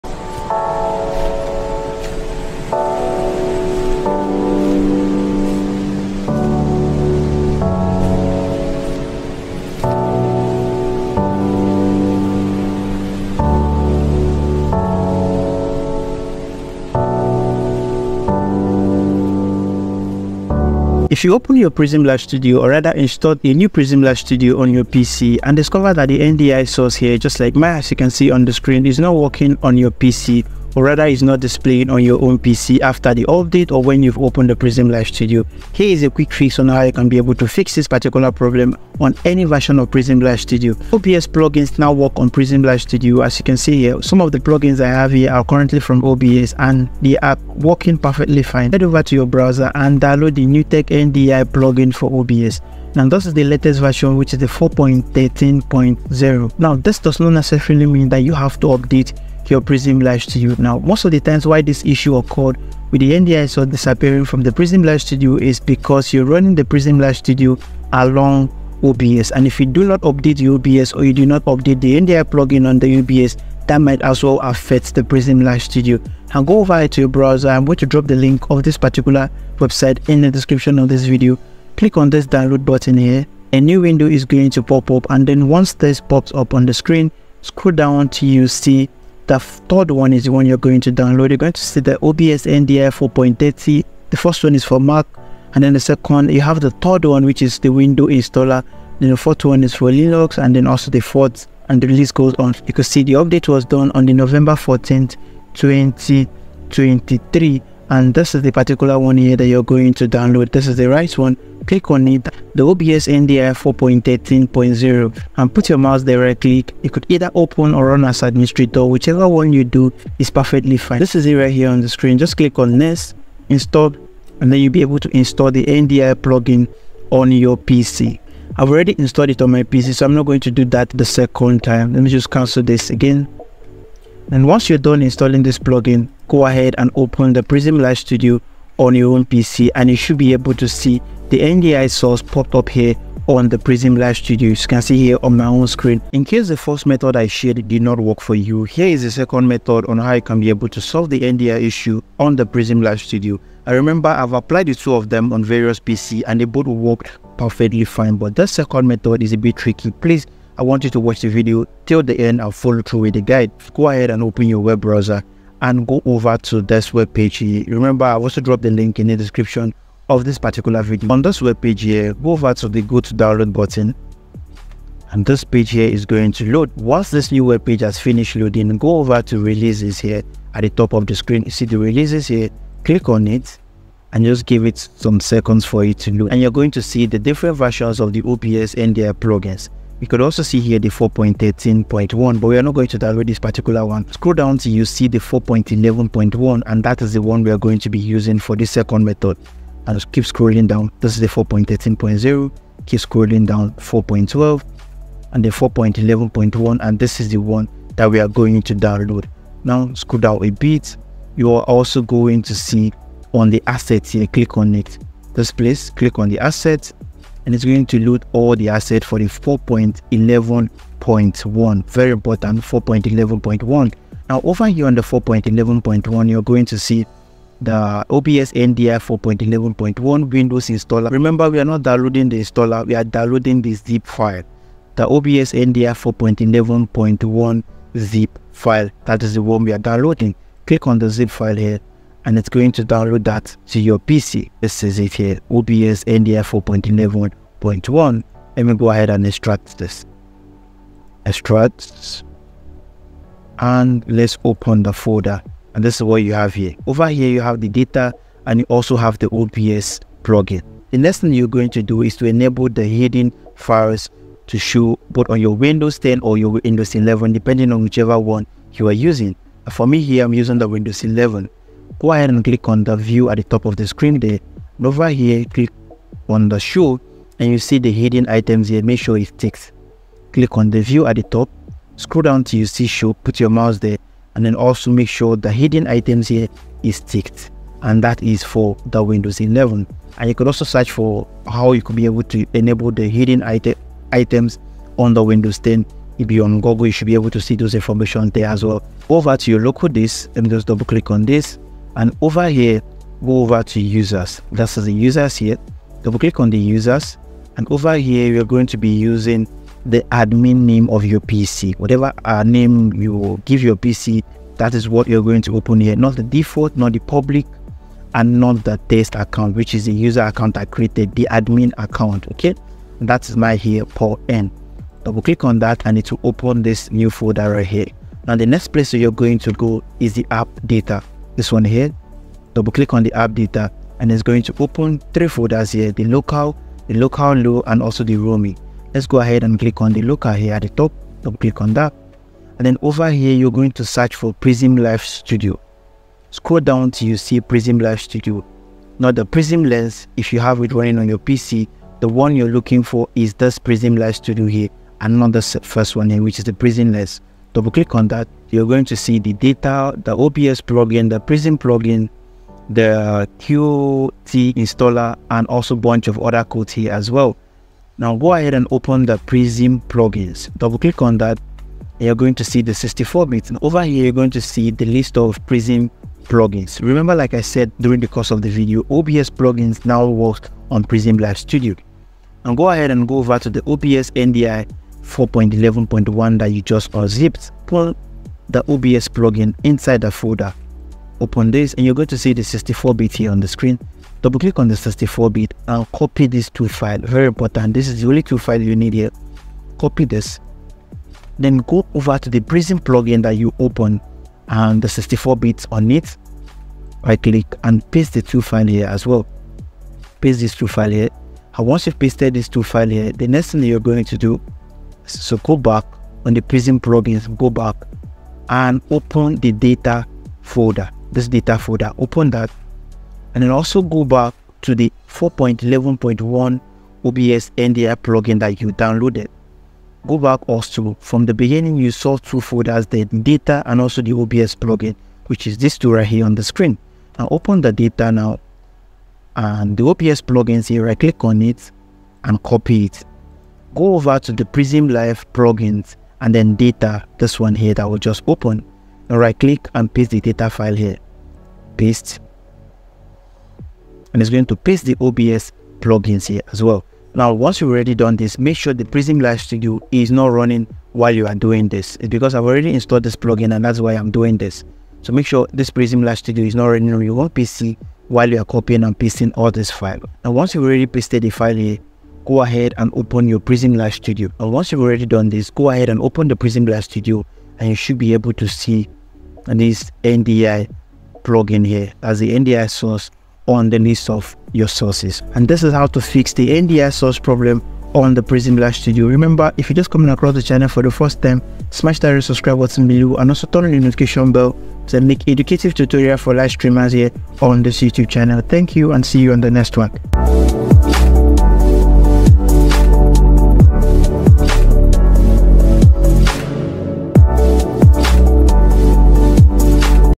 Easy Tutorial: Prism Live Studio NDI Plugin Fixed | Download & Install
If You have ever opened or downloaded Prism Live Studio upon install discovered that the NDI Plugin is not displaying, then you should watch this video. Cause it enlightens you on how to fix Prism Live studio NDI error just by downloading this plugin in the video.